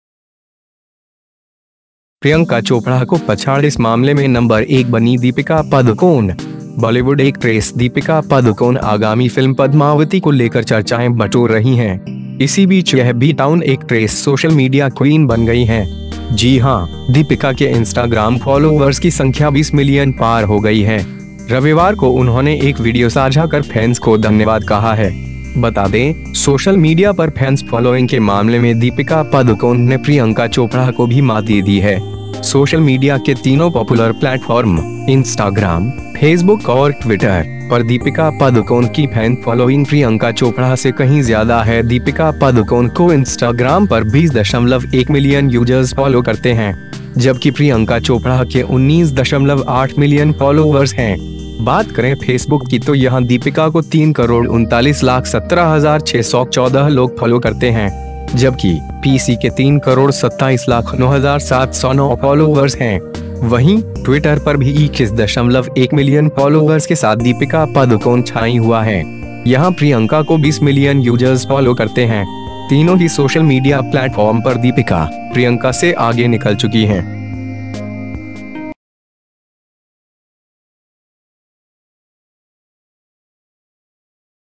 प्रियंका चोपड़ा को पछाड़ इस मामले में नंबर 1 बनीं दीपिका पादुकोण
बॉलीवुड एक्ट्रेस दीपिका पादुकोण आगामी फिल्म 'पद्मावती' को लेकर चर्चाएं बटोर रही हैं. इसी बीच यह बी-टाउन एक्ट्रेस सोशल मीडिया क्वीन बन गई हैं. जी हां, दीपिका के इंस्टाग्राम फॉलोवर्स की संख्या 20 मिलियन पार हो गई हैं. रविवार को उन्होंने एक वीडियो साझा कर फैन्स को धन्यवाद कहा है. बता दें, सोशल मीडिया पर फैन्स फॉलोइंग के मामले में दीपिका पादुकोण ने प्रियंका चोपड़ा को भी मात दे दी है. सोशल मीडिया के तीनों पॉपुलर प्लैटफॉर्म (इंस्टाग्राम, फेसबुक और ट्विटर) पर दीपिका पादुकोण की फैन फॉलोइंग प्रियंका चोपड़ा से कहीं ज्यादा है.दीपिका पादुकोण को इंस्टाग्राम पर 20.1 मिलियन यूजर्स फॉलो करते हैं. जबकि प्रियंका चोपड़ा के 19.8 मिलियन फॉलोवर्स हैं. बात करें फेसबुक की तो यहां दीपिका को 33,917,614 लोग फॉलो करते हैं. जबकि, पीसी के 32,709,709 फॉलोवर्स हैं. वहीं, ट्विटर पर भी 21.1 मिलियन फॉलोवर्स के साथ दीपिका पादुकोण छाईं हुआ हैं. यहां प्रियंका को 20 मिलियन यूजर्स फॉलो करते हैं. तीनों ही सोशल मीडिया प्लैटफॉर्म पर दीपिका, प्रियंका से आगे निकल चुकी हैं.